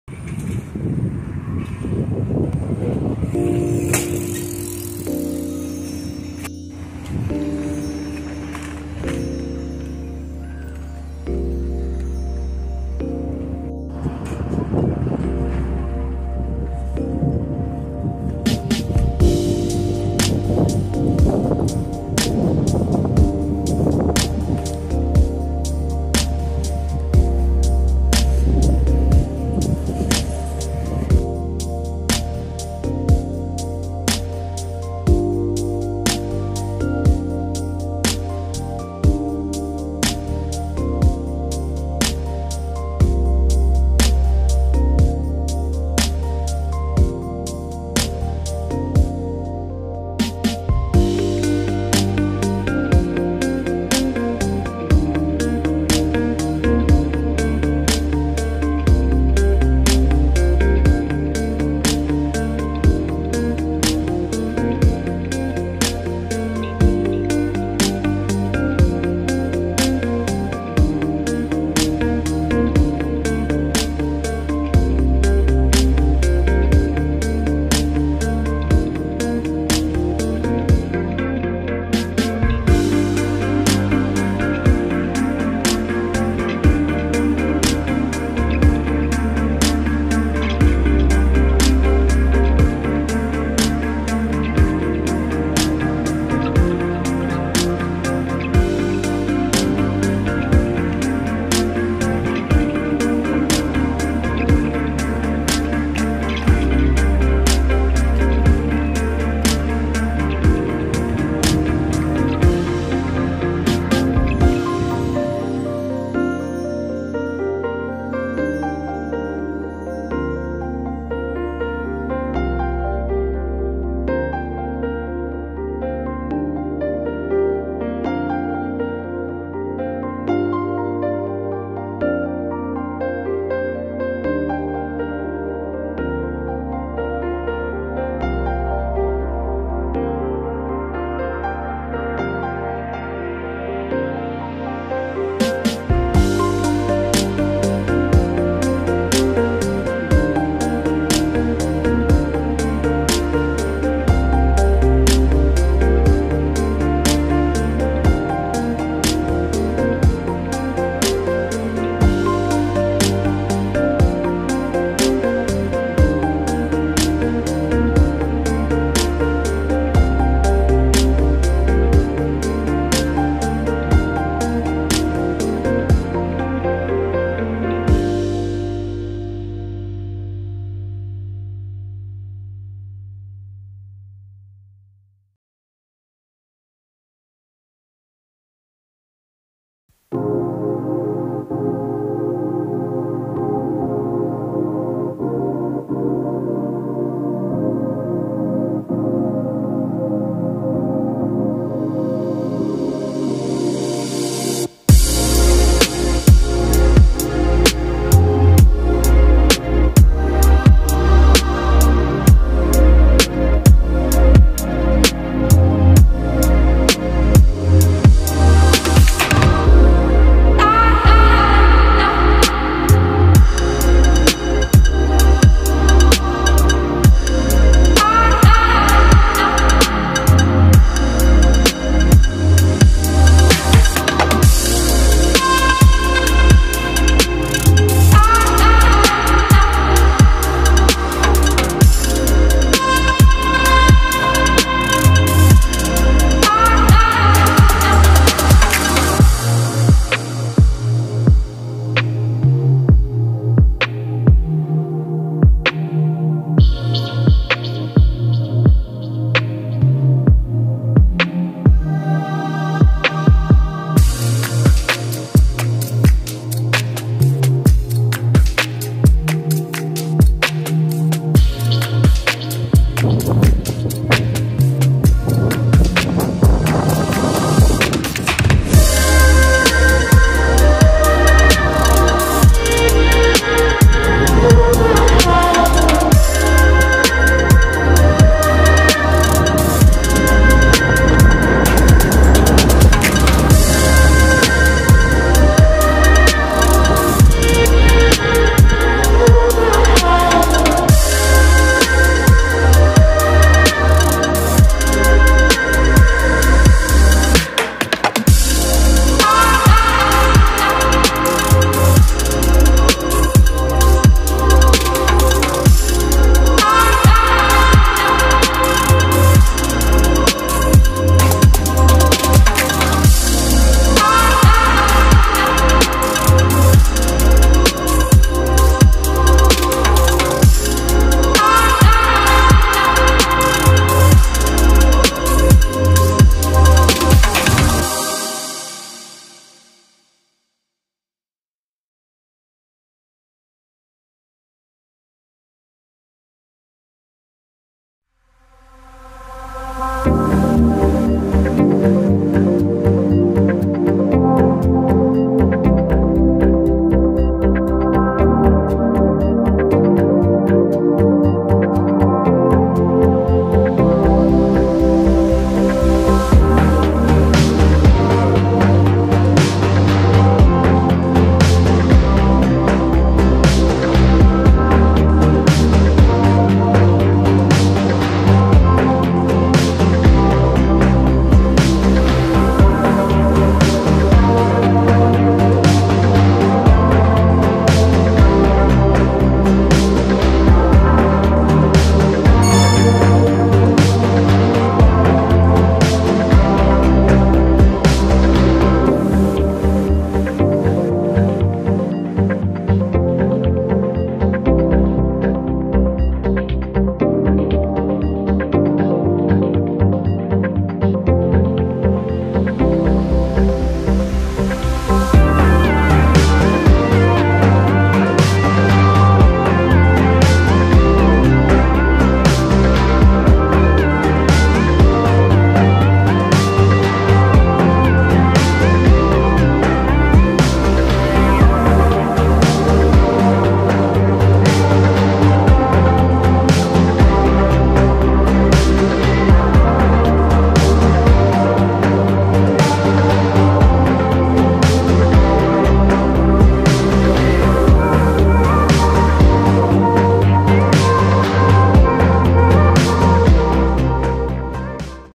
MACORON TV: Episode 41 | Walking in Hikarigaoka (Tokyo, Japan) | 東京都練馬区光が丘を歩く | 世界ふれあいマコ歩き
MACORON TV Ep 41

日曜の夕方、練馬区光が丘を歩きました。